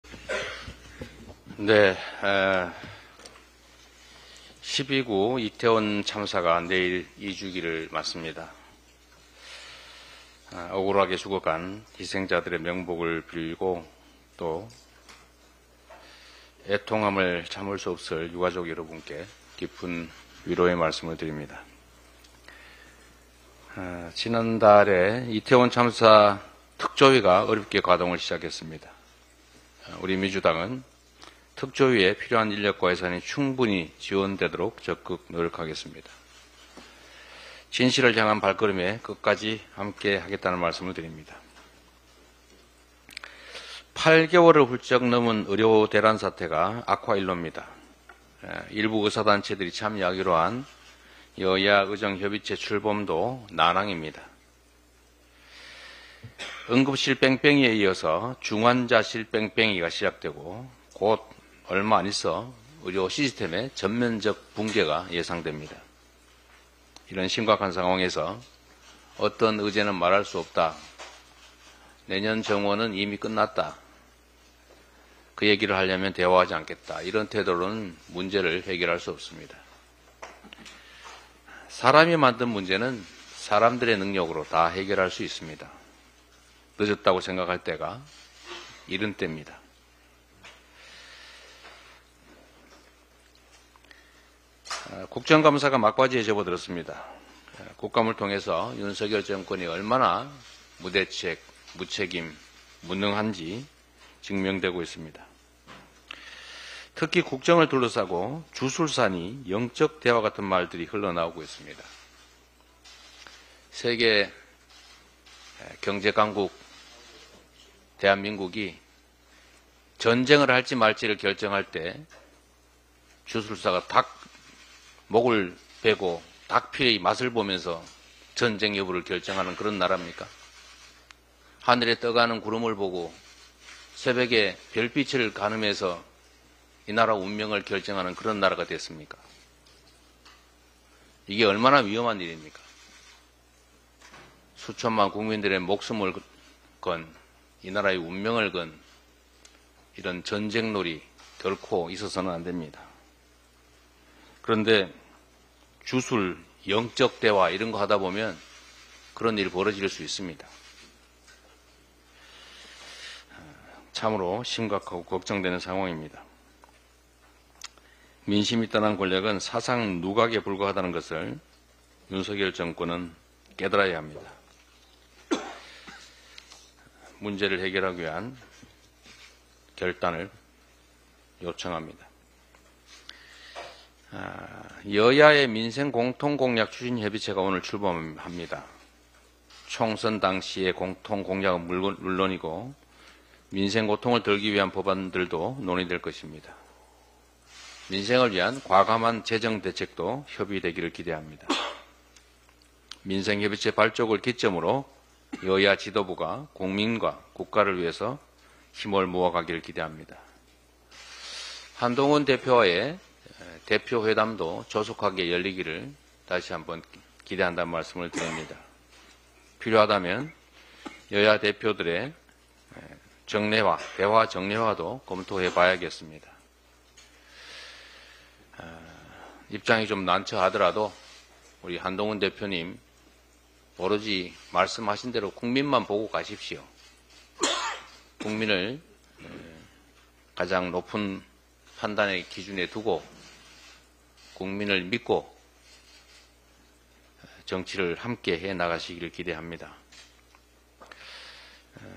"국정에 주술사·영적 대화라니..." 무대책·무책임·무능 정권 강력 비판... 이재명 "이게 나라냐??"
28일 오전 서울 여의도 국회에서 더불어민주당 최고위원회의가 열렸다. 이 영상은 이재명 대표 발언 담고 있다.

(영상 : 권민구 기자, 윤다림 PD)